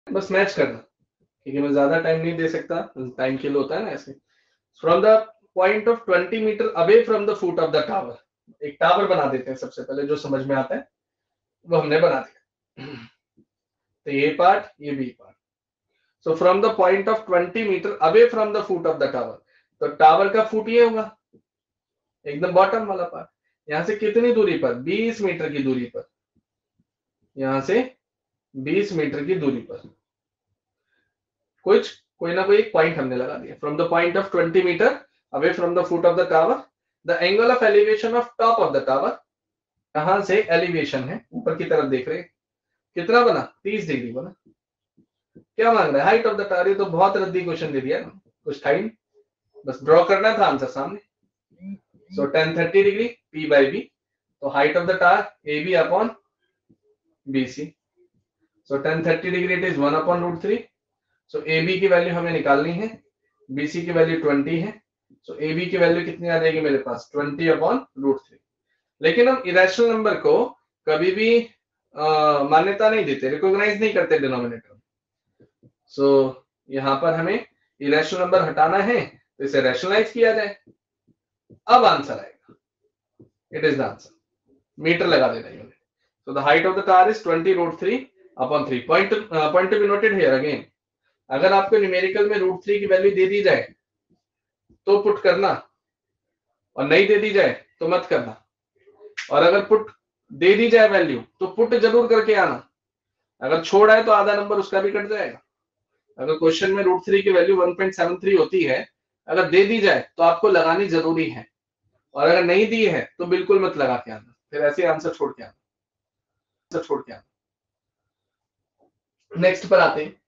Trigonometry | Important Question | Class 10 | Previous Year
Questions
From a point 20 m away from the foot of a tower, the angle of elevation of the top of the tower is 30 Degree. Find the height of the tower.